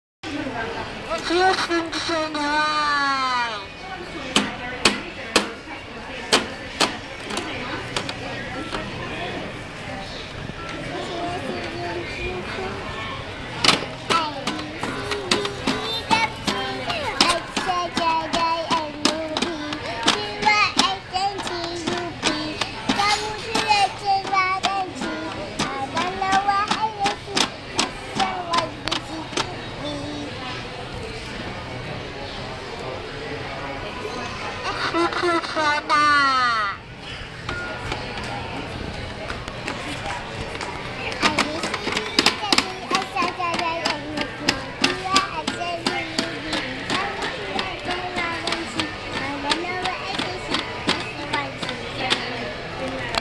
Singing At The Shops
Little B singing at the shops...